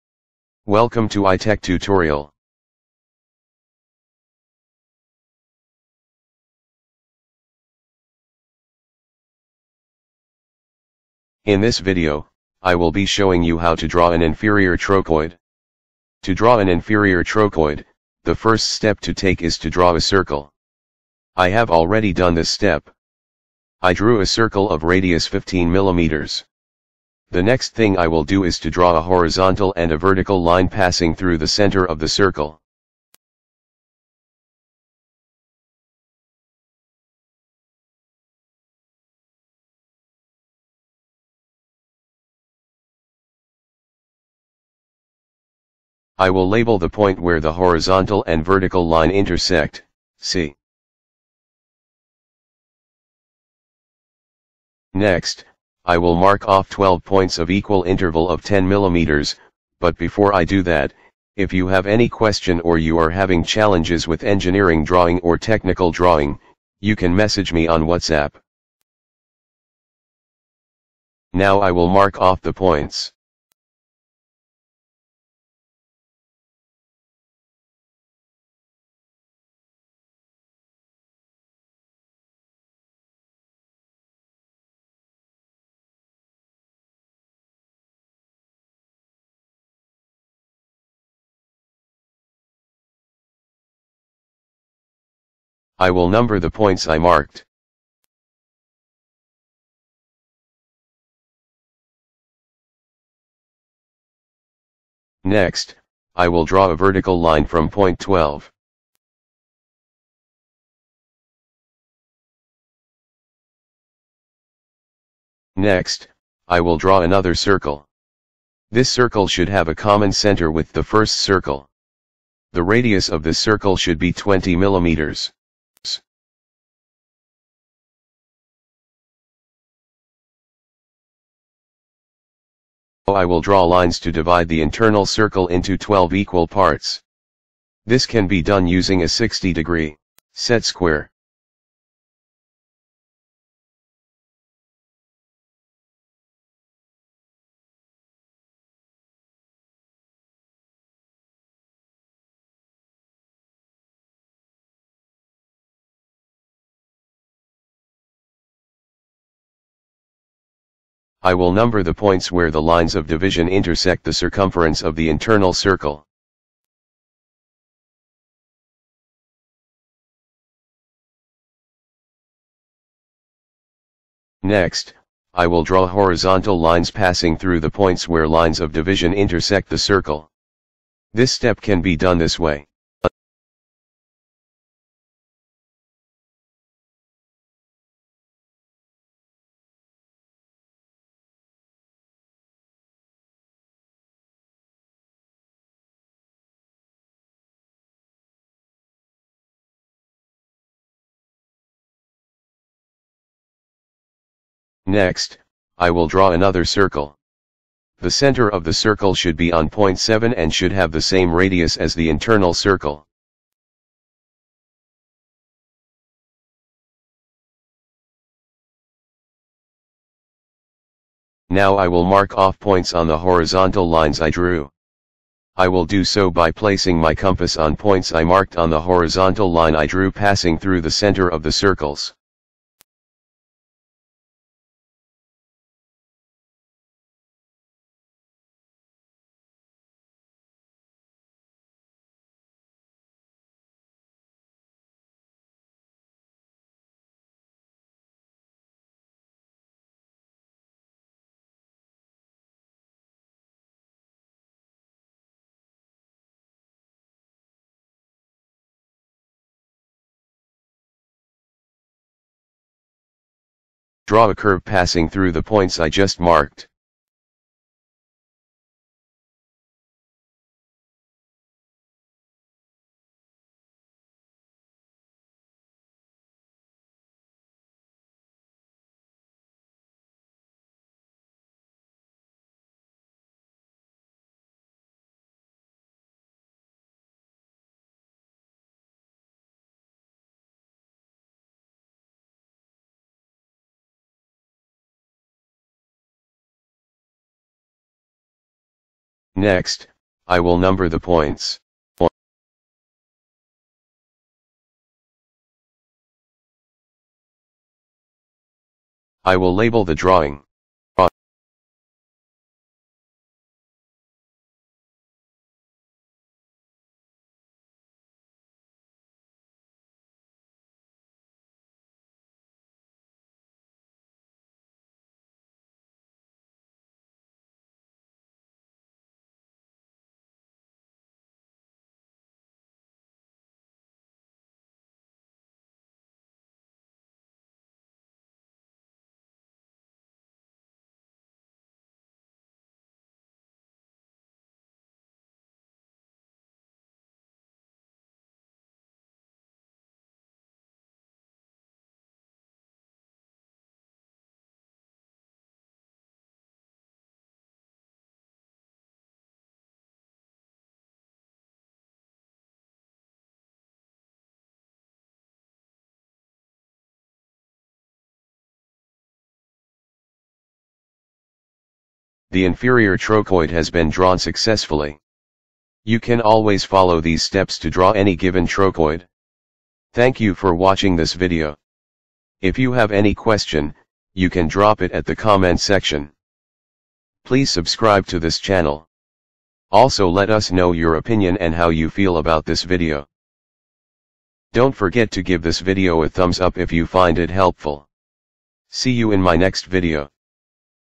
HOW TO DRAW AN INFERIOR TROCHOID || loci || engineering drawing || technical drawing
This video explains step by step how to draw an inferior trochoid in a simple and understandable way.

Instrument  Used
Pencil
Ruler
Compass
Set-square
T-square
French curve